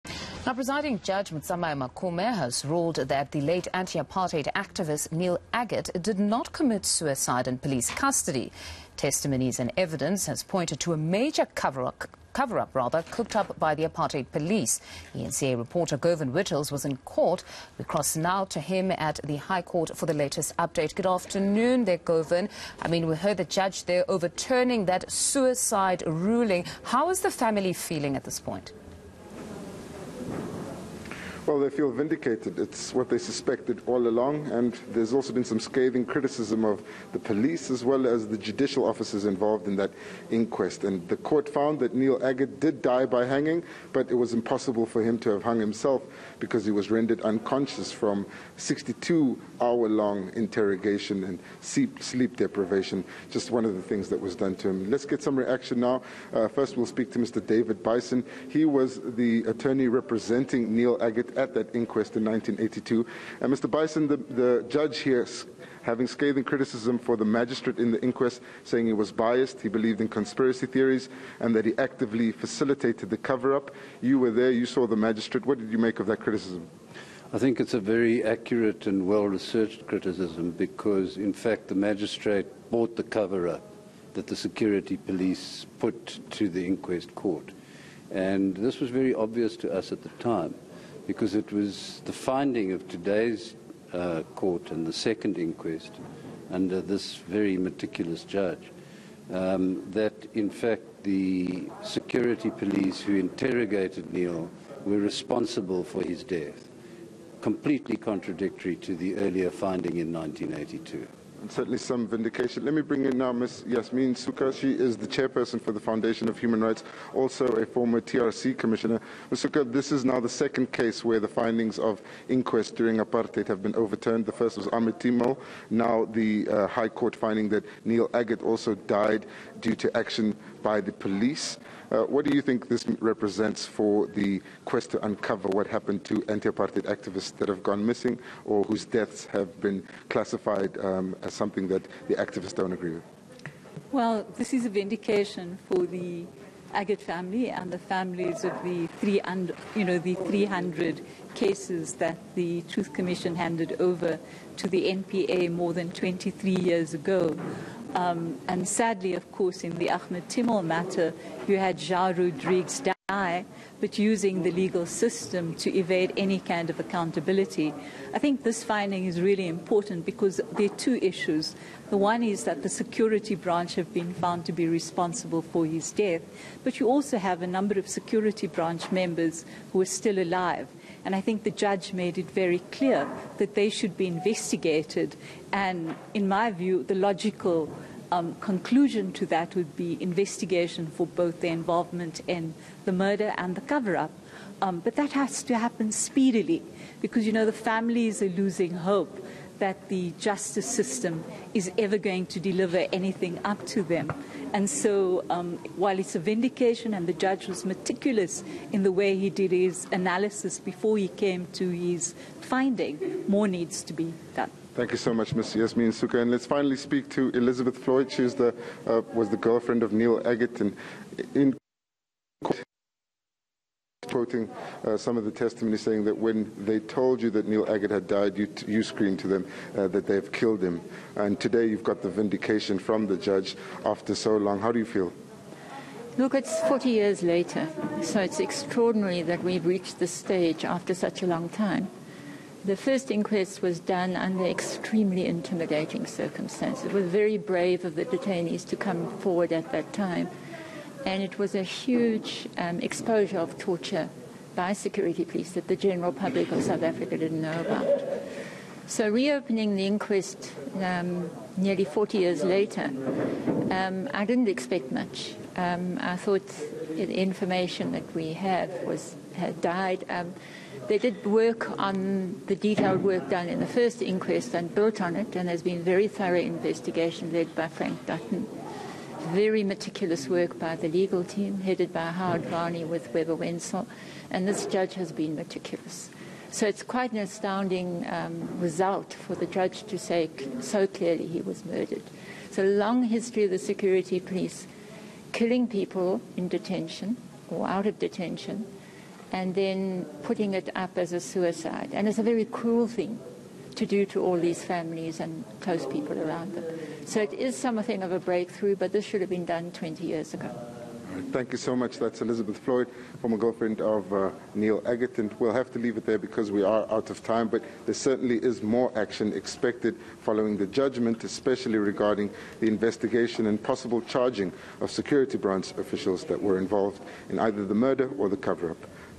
Judge rules Neil Aggett's death not suicide
Presiding Judge Motsamai Makume has ruled that the late anti-apartheid activist Neil Aggett did not commit suicide in police custody. eNCA reporter Govan Whittles was in court and the details.